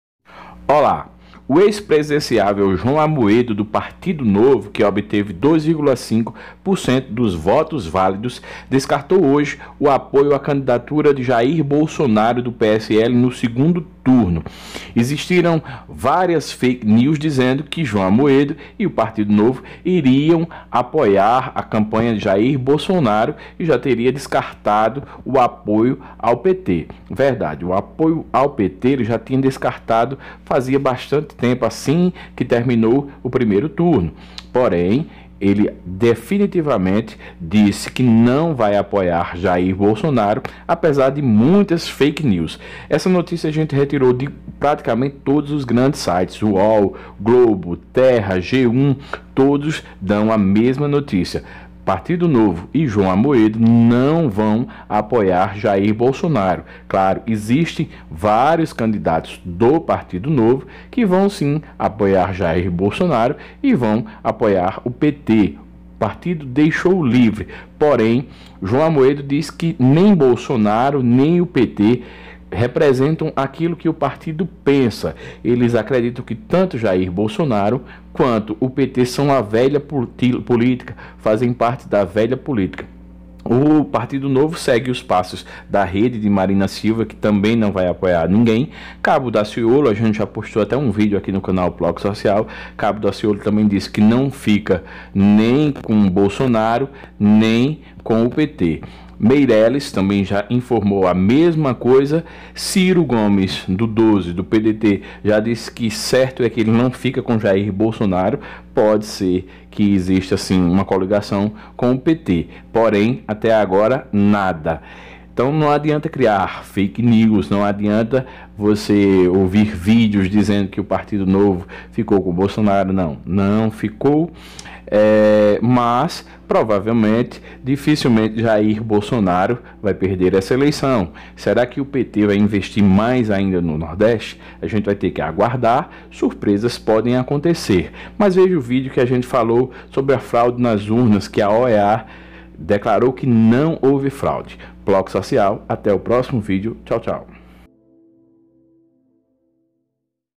PARTIDO NOVO DESCARTA APOIO A JAIR BOLSONARO E PT: "ELES SÃO A VELHA POLITICA"
O lider do PartidoNovo, JoãoAmoedo descartou apoiar Jair Bolsonaro ou PT no segundo turno das eleições, para ele esses partidos representam a velha politica.

Descarte FakeNews, essas informações foram retiradas dos maiores portais brasileiros.